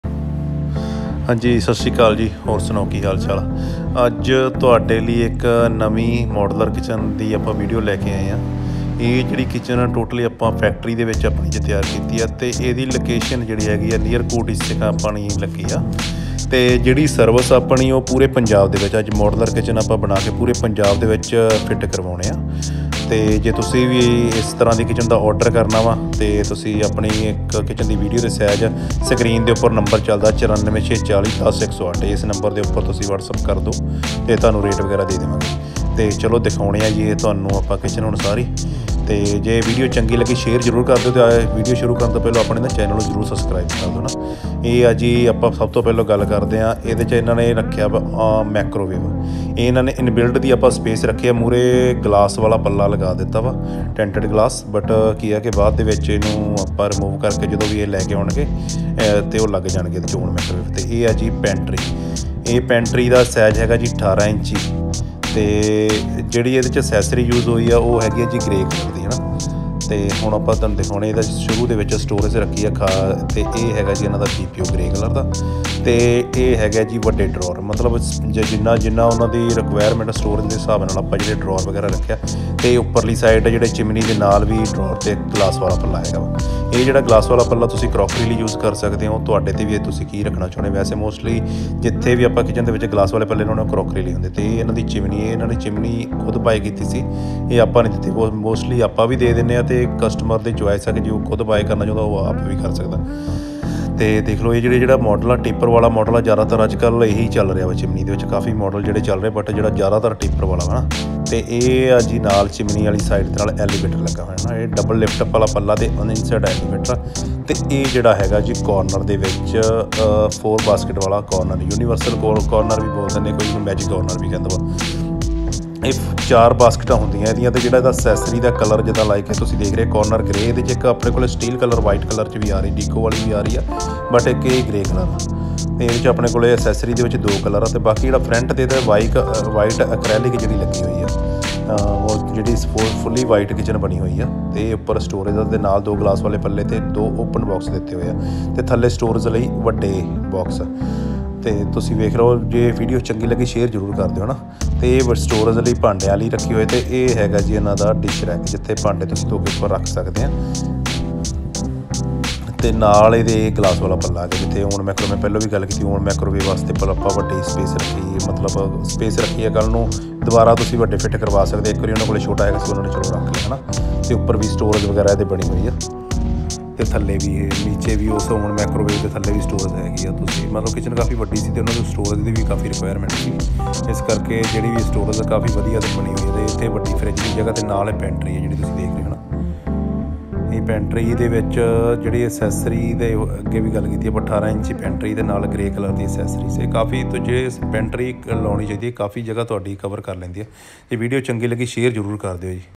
white moduler kitchen | near kot ise kha | JOHAL KITCHEN
HNJI KIDAA OH SAREY | AJJ NVI KITCHEN LAI K AYE A JI NEAR KOT ISE KHA KOL | JIVE K TUCI VIDEO VICH DEKHII KITCHEN |TE TUHANU SADII VIDEO KIDA DI LAGGI APNE VICHAR COMMENT  BOX VICH JROOR SHARE KARYIO |

THANKS||